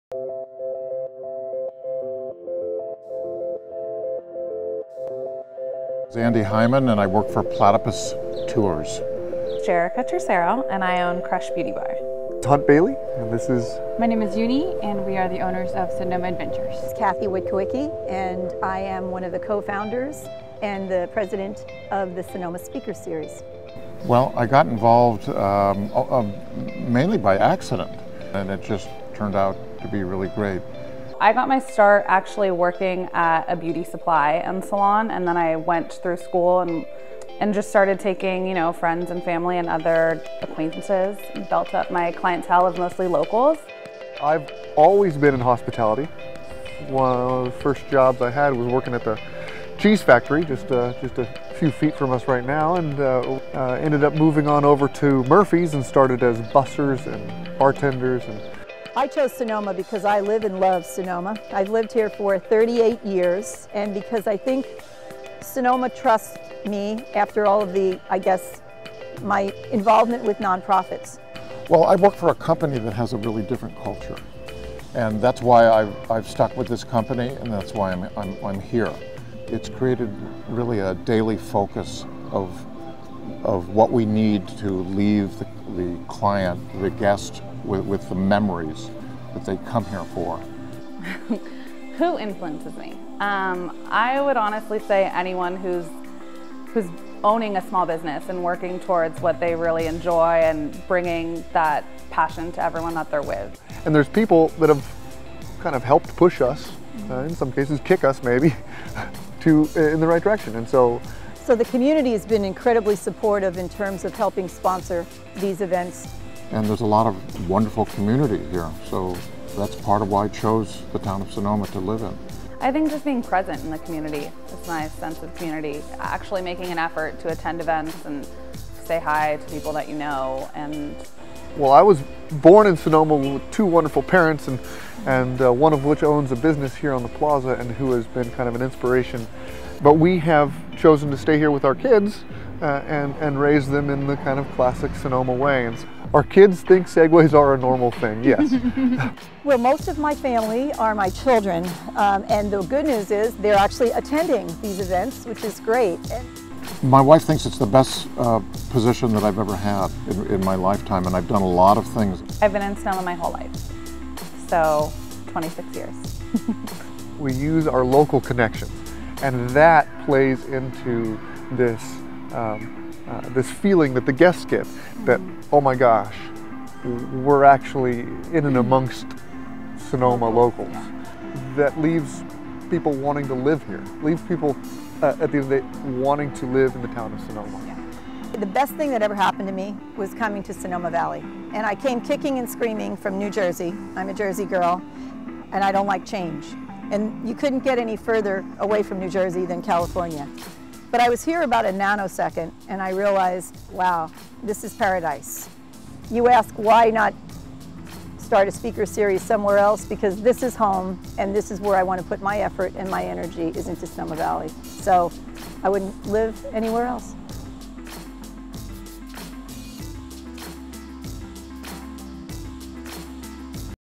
Sonoma Valley's Faces of Travel
National Travel and Tourism Week 2017 is all about celebrating the power of travel, and particularly the people who make our industry remarkable. To get in the spirit, Sonoma Valley is highlighting some of our own “faces of U.S. Travel.”